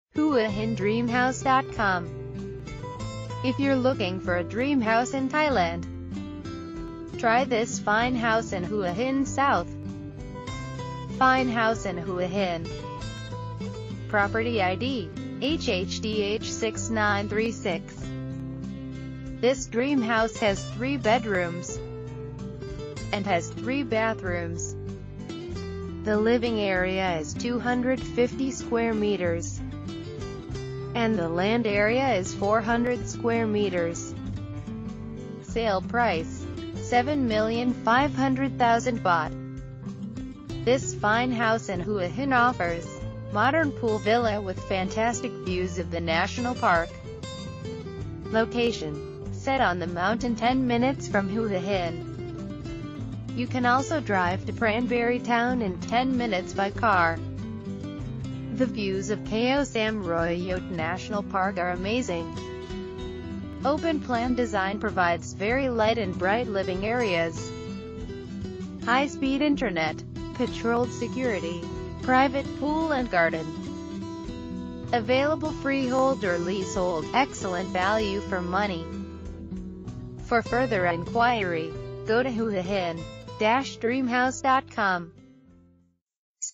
Fine House for Sale in Hua Hin HHDH6936
12/249 Hua Don Village
Nong Khae, Amphoe Hua Hin
Prachuap Khiri Khan 77110
Thailand

HuaHin-DreamHouse.com
If you’re looking for a dream house in Thailand?
Try this Fine House in Hua Hin South

Fine House in Hua Hin
Property ID: HHDH6936
This Dream House has 3 bedrooms
And has 3 bathrooms
The Living Area is 250 m²
And the Land Area is 400 m²
Sale Price: 7500000 Baht

This Fine House in Hua Hin offers:
Modern pool villa with fantastic views of the national park
Location: set on the mountain 10 minutes from Hua Hin
You can also drive to Pranburi Town in 10 minutes by car
The views of Khao Sam Roi Yot national park are amazing
Open-plan design provides very light and bright living areas
High speed Internet, patrolled security, private pool and garden
Available freehold or leasehold - excellent value for money